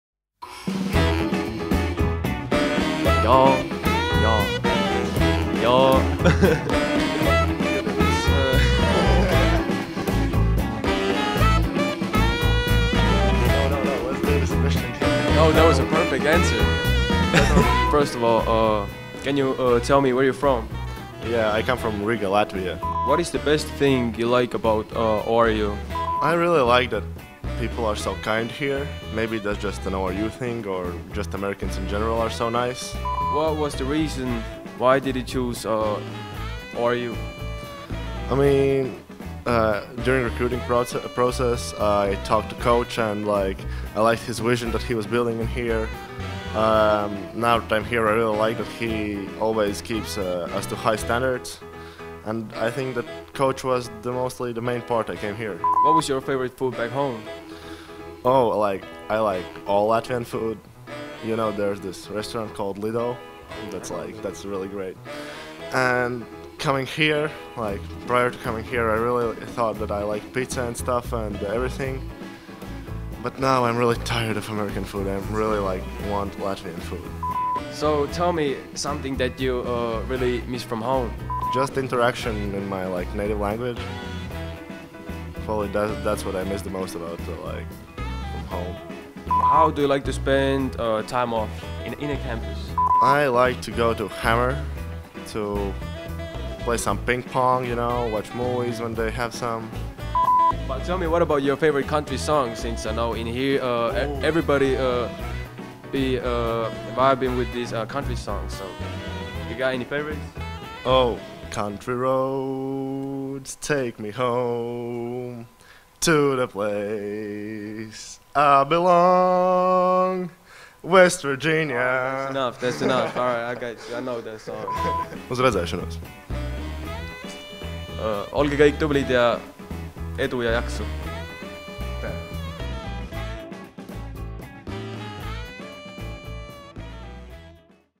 Meet the Golden Eagles - Francis Lācis
Meet ORUMBB freshman Francis Lācis in the third edition of "Meet the Golden Eagles" with host Carlos Jürgens.